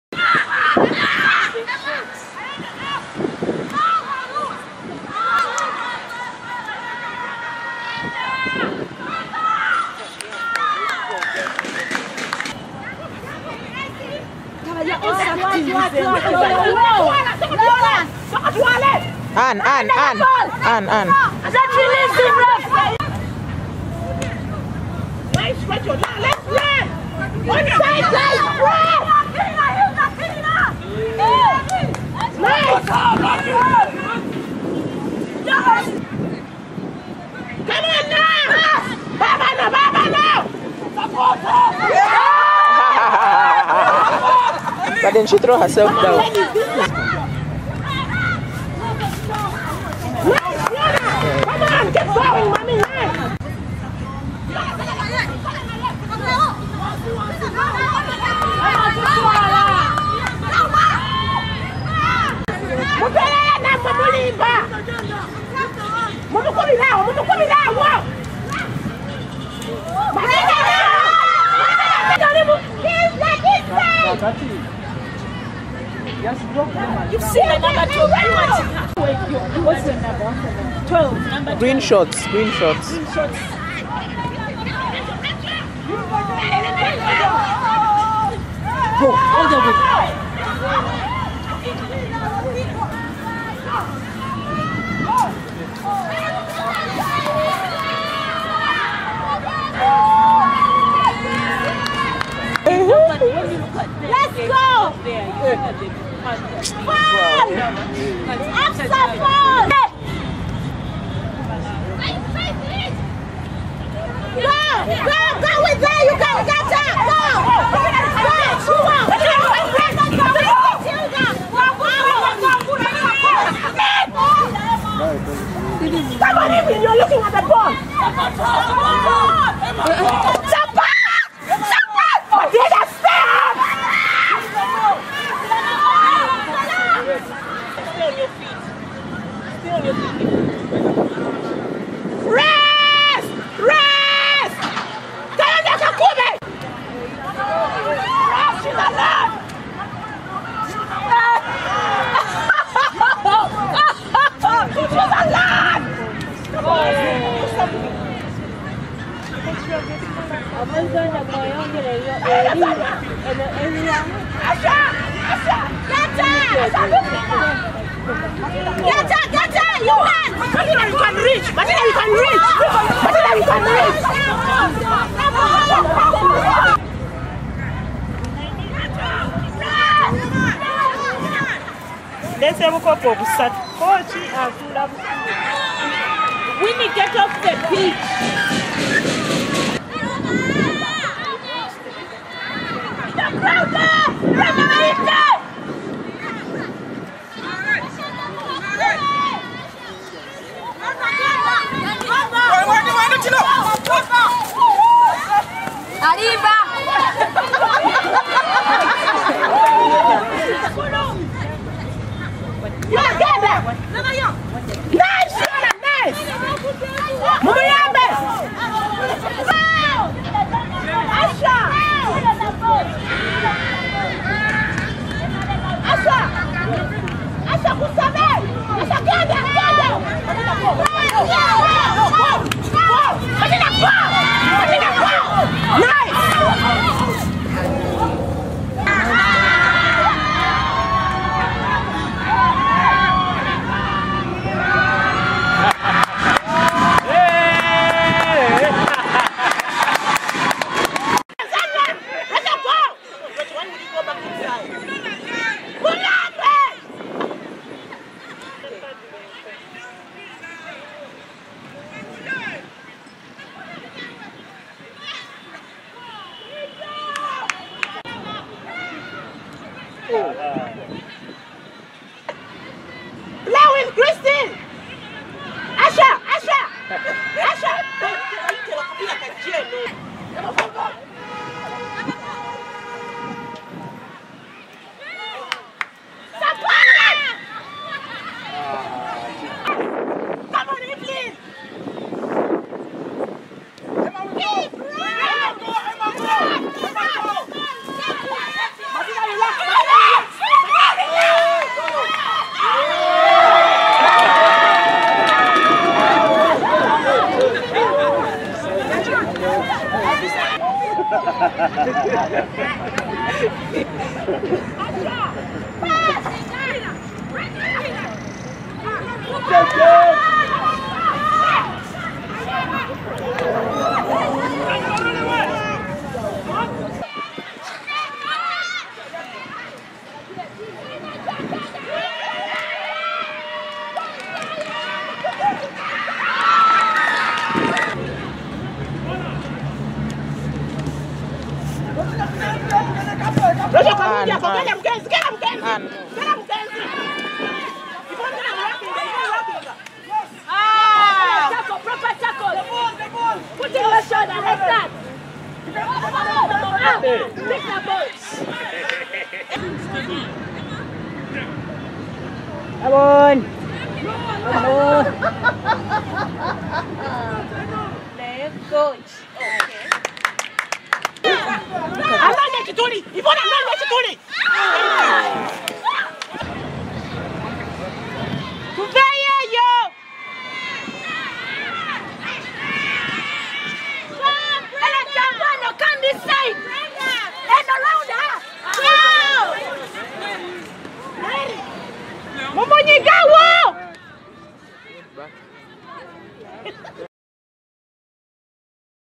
Highlights Thunderbirds versus Katogo Pythons - 31052014
Kyadondo's Thunderbirds (blue) hosted Katogo Pythons (white - team made up of players from clubs in Entebbe, Jinja, Kampala and Mbale) in a second 15 aside game of rugby.  Thunderbirds won 27-05.